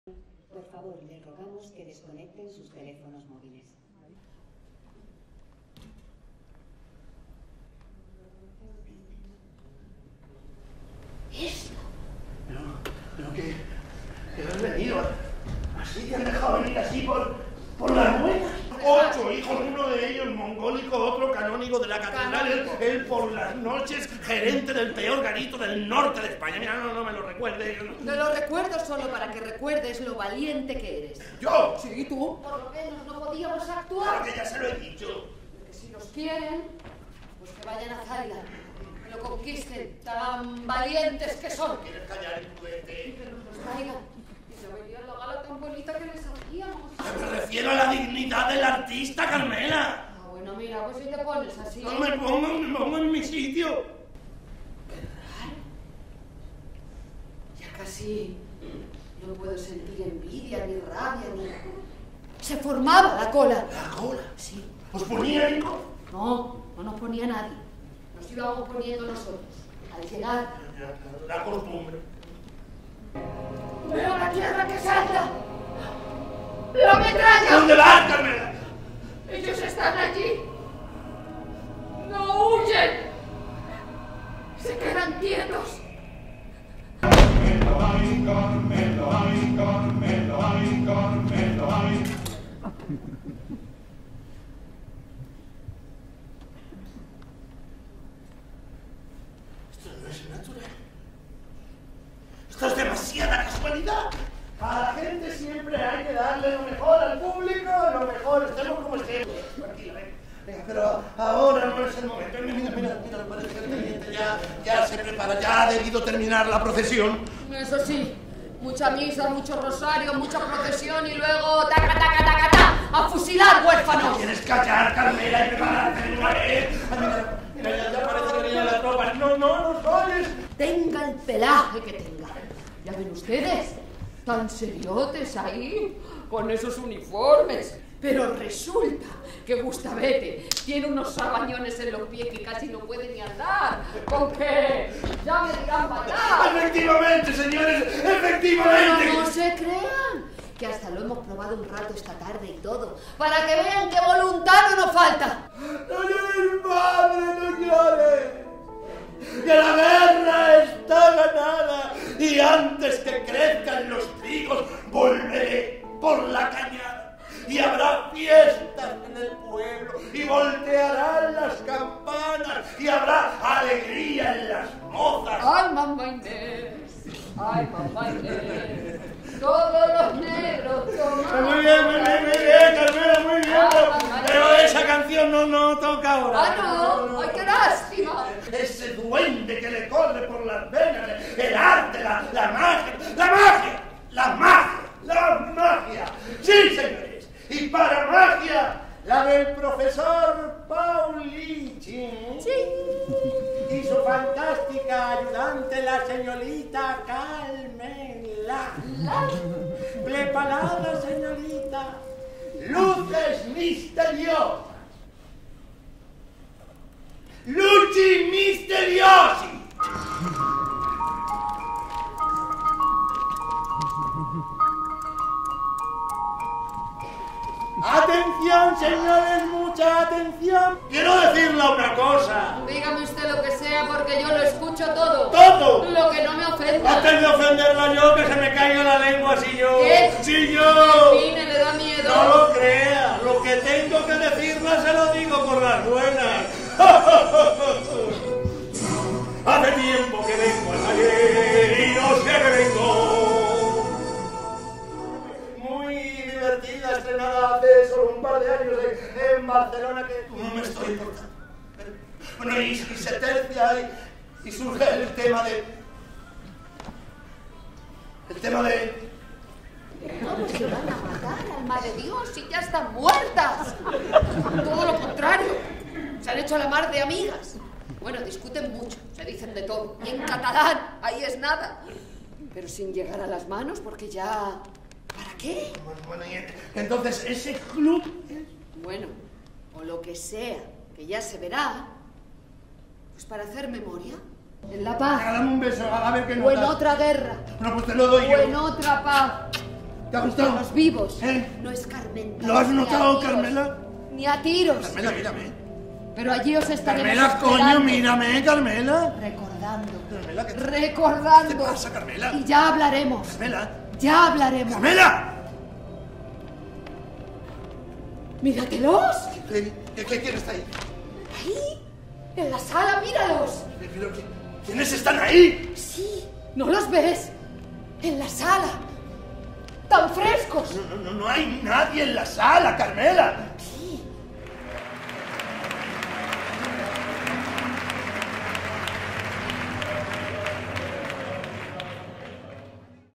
AY CARMELA
Producciones Faraute y Pentación Espectáculos

presentan

Dramaturgia y dirección José Carlos Plaza

Del 5 de abril al 18 de junio de 2023

El exitoso texto de José Sanchis Sinisterra, estrenado en 1987 y por el que su autor recibió el Premio Max a la mejor autoría teatral en 1999, llega al teatro Bellas Artes de Madrid tras un inicio de gira con el que ya están cosechando numerosos éxitos. Pepón Nieto (EL EUNUCO, LA COMEDIA DE LAS MENTIRAS) y Maria Adánez (INSOLACION, EL PEQUEÑO PONI , LULÚ) personifican a Paulino y Carmela, bajo la dirección del prestigioso y reconocido director José Carlos Plaza (HÉCUBA, MEDEA, DIVINAS PALABRAS ).